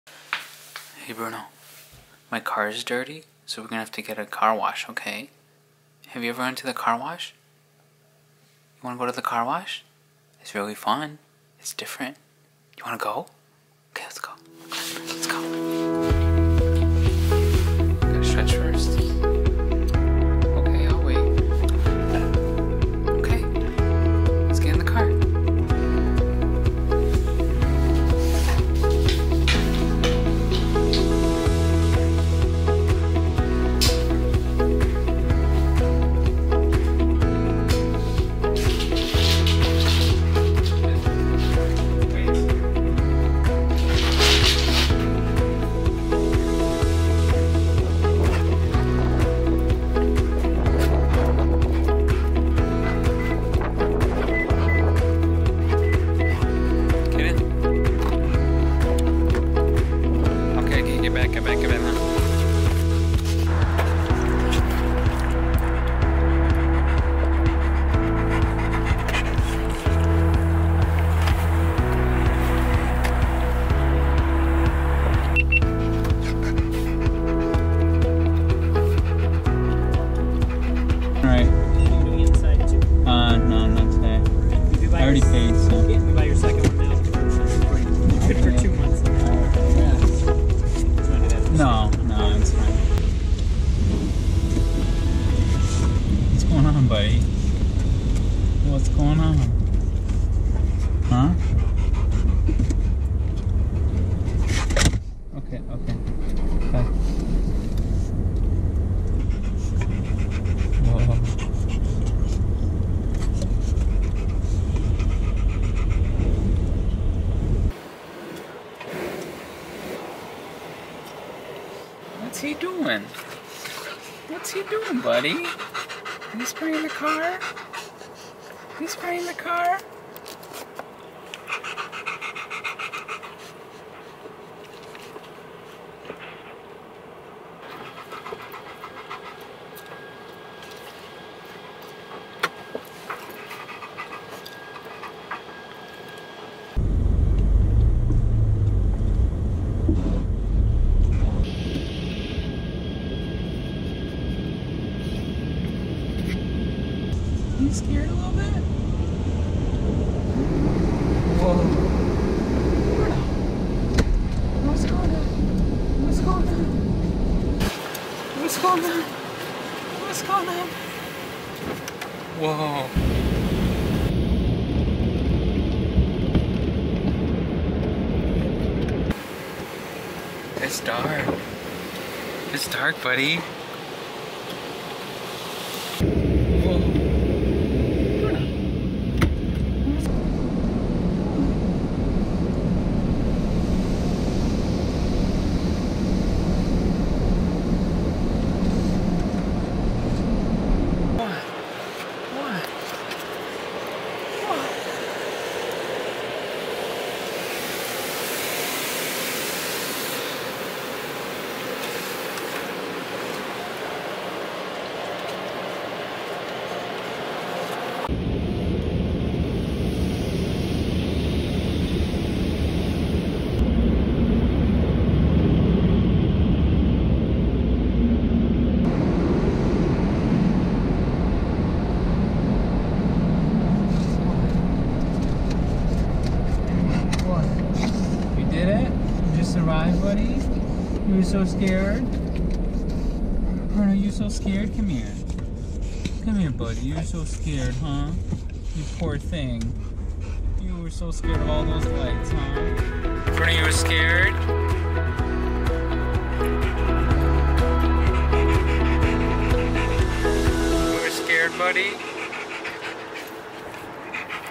Rottweiler's first time at a car wash!|58
Bruno went to a car wash. Lets just say he will not be going back again.